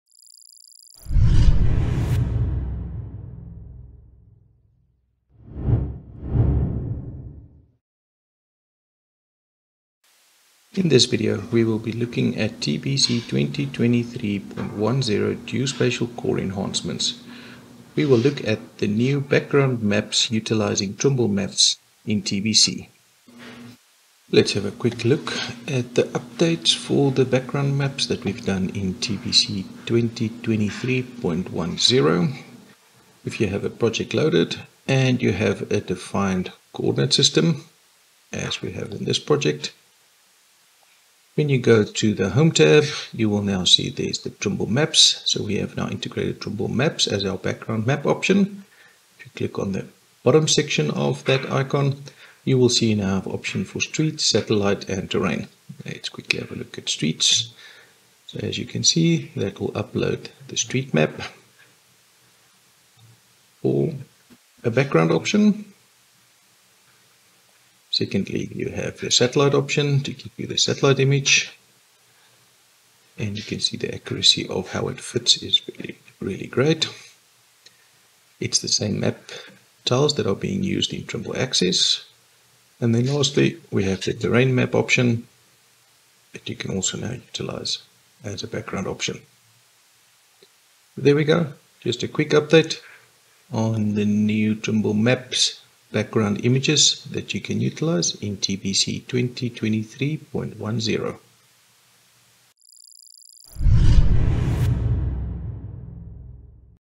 TBC 2023.10 Trimble Maps
This video highlights the new “Trimble Maps” command available within the Home/View tab in TBC 2023.10 and later. Use the Trimble Maps command to show/hide any available map data retrieved through Trimble Map Services associated with the objects in your project.
For questions, contact your local Trimble representatives and distributors.